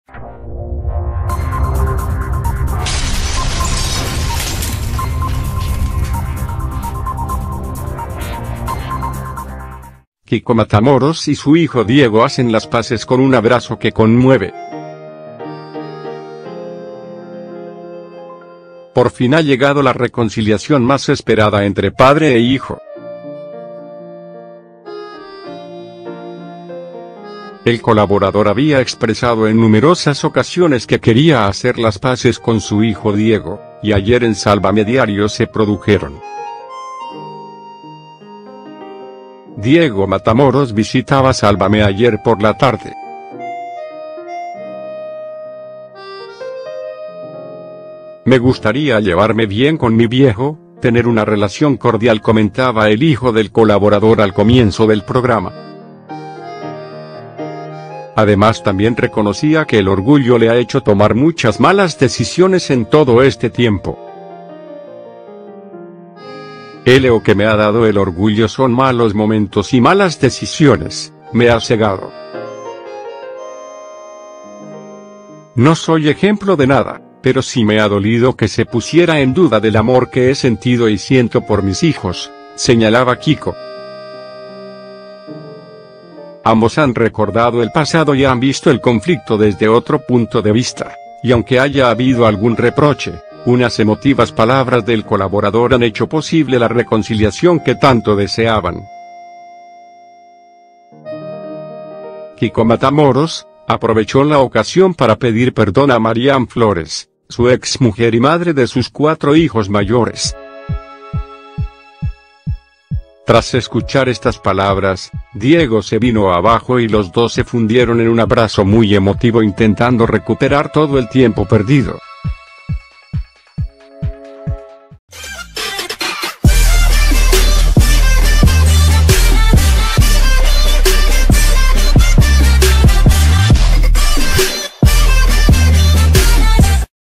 Kiko Matamoros y su hijo Diego hacen las paces con un abrazo que conmueve
Por fin ha llegado la reconciliación más esperada entre padre e hijo.